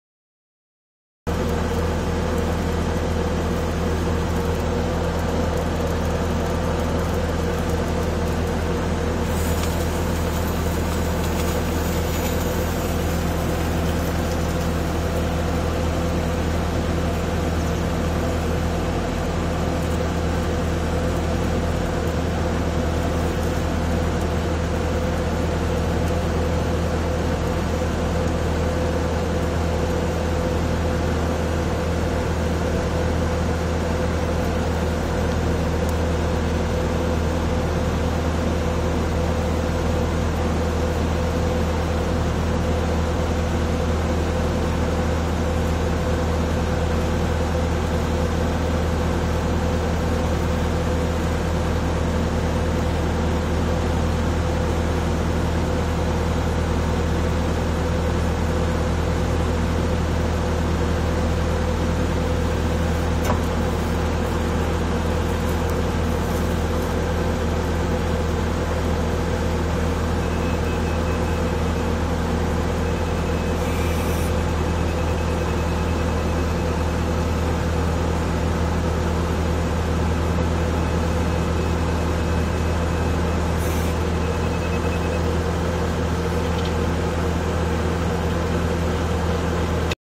Ruuvi paalun kairaus
Bobcat A300 kairaa ruuvipaalun